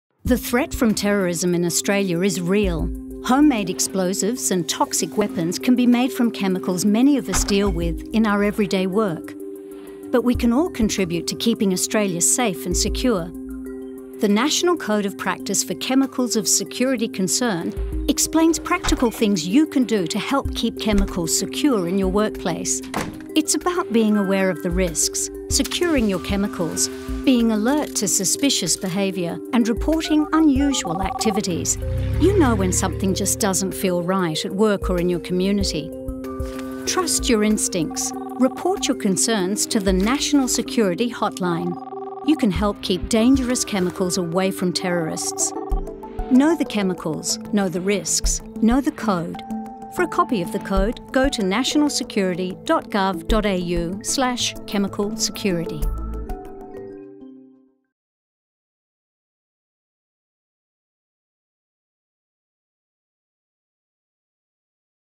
Overview of Australia’s Chemical Security Programme
This video provides advice to the community on how to help keep Australia safe from terrorism and encourages reporting of suspicious behaviour to the National Security Hotline on .